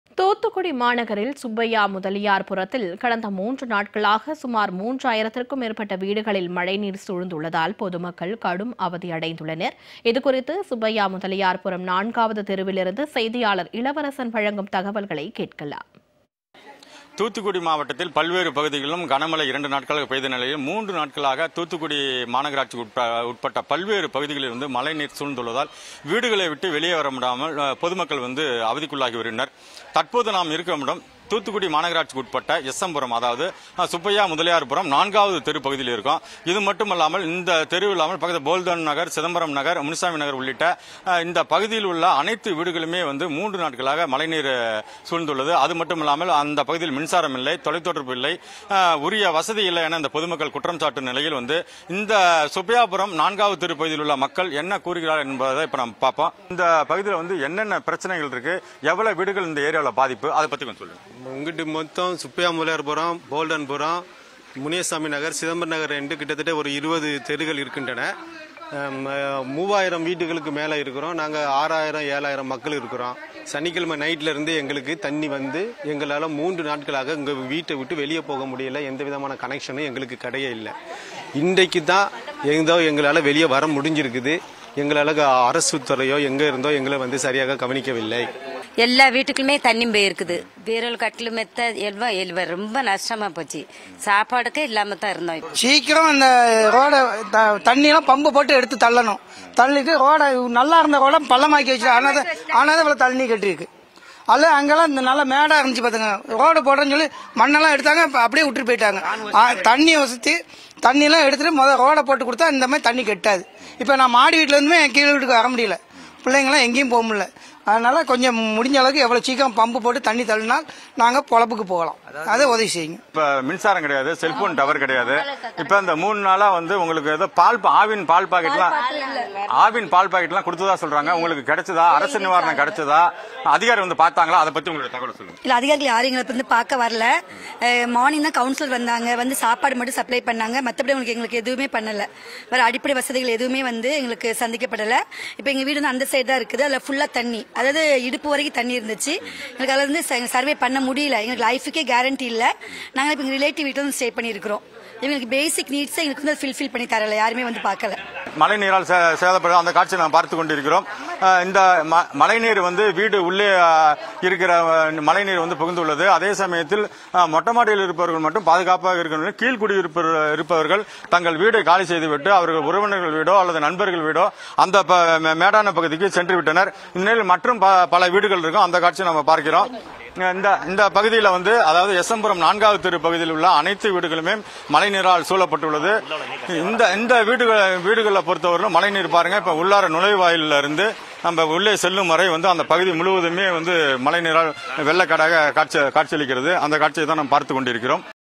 3 நாட்களாக வீடுகளை சூழ்ந்த வெள்ள நீர் நடவடிக்கை எடுக்காத அரசு | Thoothukudi | Rain | Jaya Plus
3 நாட்களாக வீடுகளை சூழ்ந்த வெள்ள நீர் நடவடிக்கை எடுக்காத அரசு | Thoothukudi |  Rain | Jaya Plus